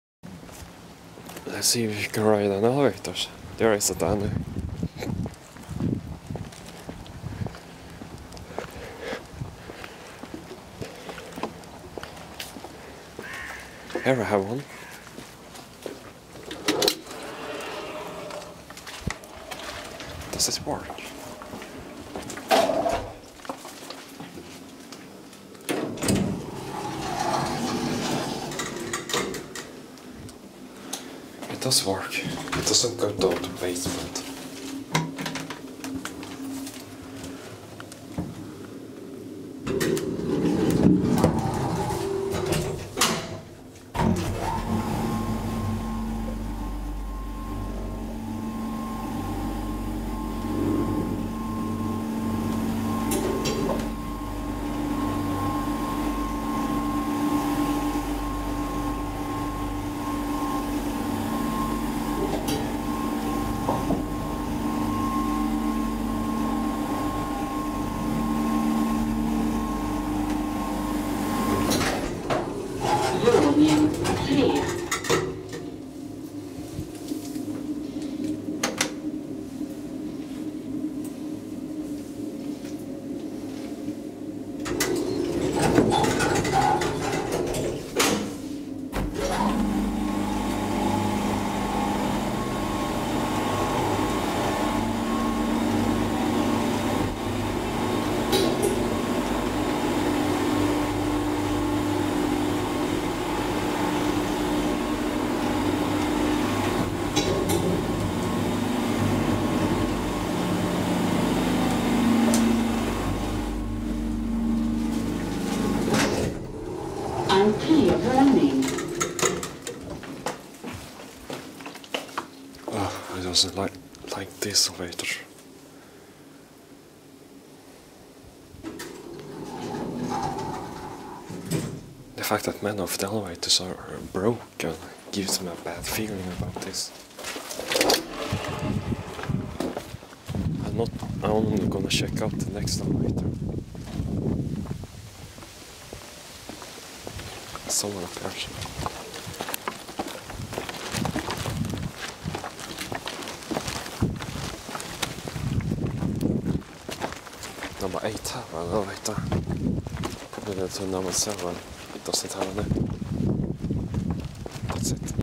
Another NTD Screw Driven elevator @ Hölövägen 9, Stavsnäs
Brand: NTD
Type: Screw-Driven
Controller: Unknown
Capacity: 5 Pers.
Floors: B, 1-3
Build no.
Location: Hölövägen 9, Stavsnäs, Värmdö, Stockholm SWEDEN